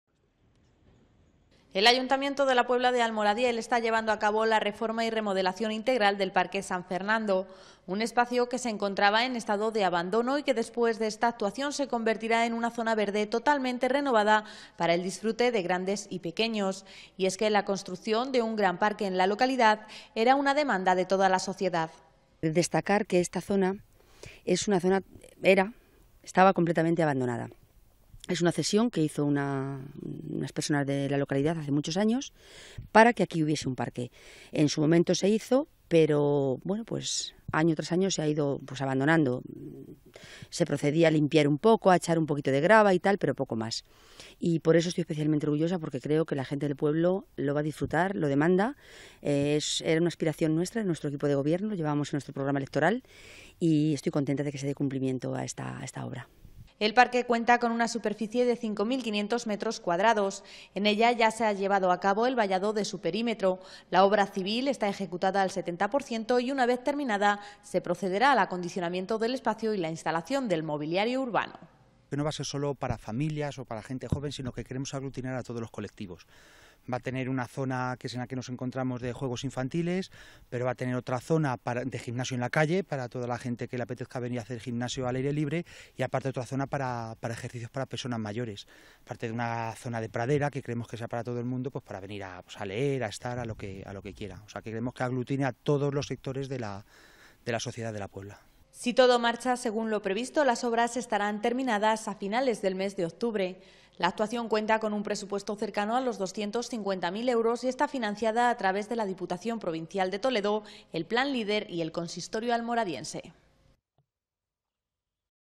La remodelación del Parque San Fernando de la Puebla de Almoradiel a buen ritmo
Las obras de remodelación integral del Parque San Fernando de la Puebla de Almoradiel marchan a buen ritmo.
Se espera que este nuevo espacio verde esté totalmente acondicionado a finales del mes de octubre.
El ayuntamiento de la Puebla de Almoradiel está llevando a cabo la reforma y remodelación integral del Parque San Fernando. Un espacio que se encontraba en estado de abandono y que después de esta actuación se convertirá en una zona verde totalmente renovada para el disfrute de grandes y pequeños. Y es que la construcción de un gran parque en la localidad era una demanda de toda la sociedad.
El parque cuenta con una superficie de 5.500 metros cuadrados. En ella ya se ha llevado a cabo el vallado de su perímetro, la obra civil está ejecutada al 70 por ciento y una vez terminada se procederá al acondicionamiento del espacio y la instalación del mobiliario urbano.
Si todo marcha según lo previsto las obras estarán terminadas a finales del mes de octubre. La actuación cuenta con un presupuesto cercano a los 250.000 euros y está financiada a través de la diputación provincial de Toledo, el plan leader y el consistorio almoradiense.